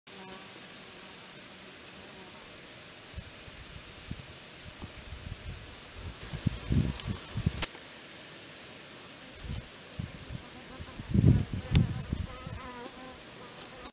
Sand Wasp, Quincy Bluff SNA, Adams Co, WI
Sand Wasp busy digging in Adams County, WI